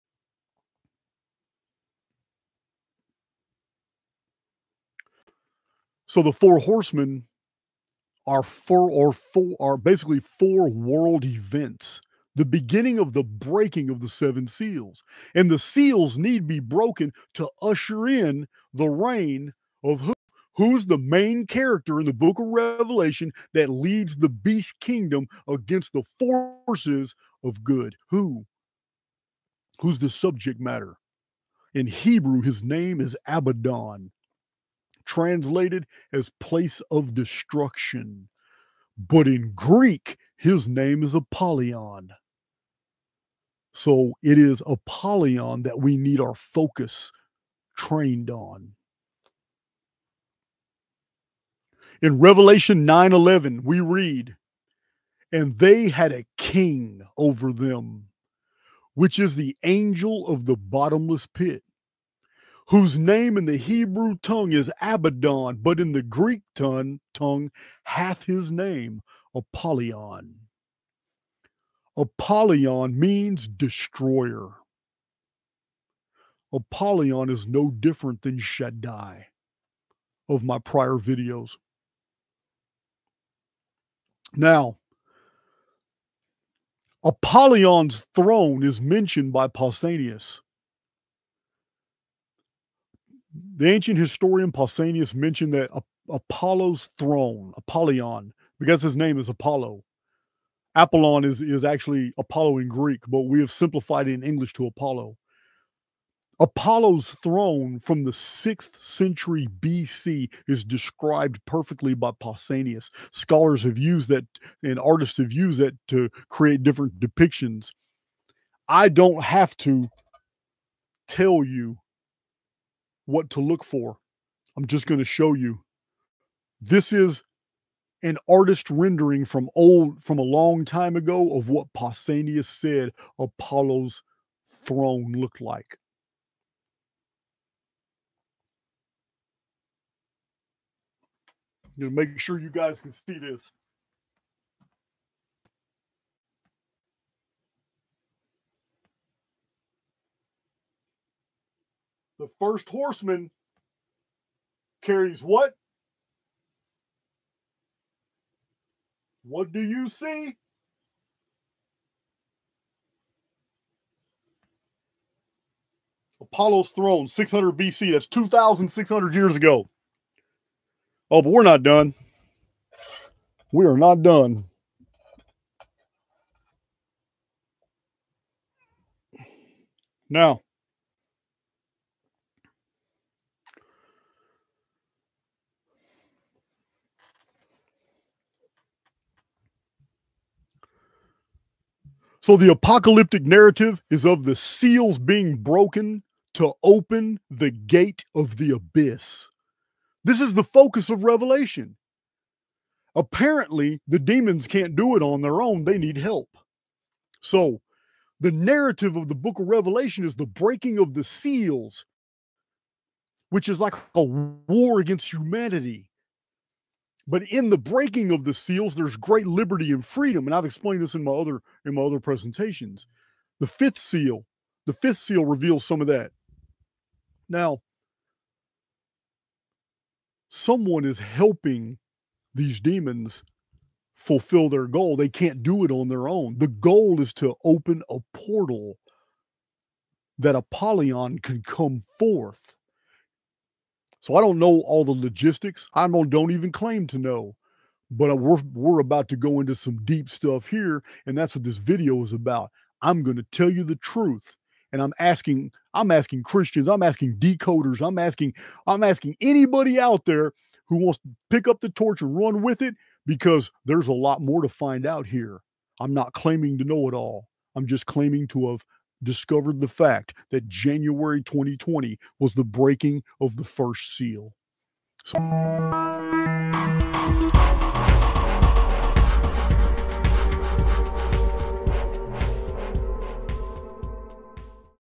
Archaix ∆ Four Horsemen Are Four World Events, Seals Must Be Broken, Abaddon, Apollo, or Apollyon...
Video Source: 26:28-31:03

Archaix ∆ Four Horsemen Are Four World Events, Seals Must Be Broken, Abaddon, Apollo, or Apollyon, The Destroyer in Greek Translation, January 2020 Was Breaking of the First Seal

~ ∆RCHAIX SHORTS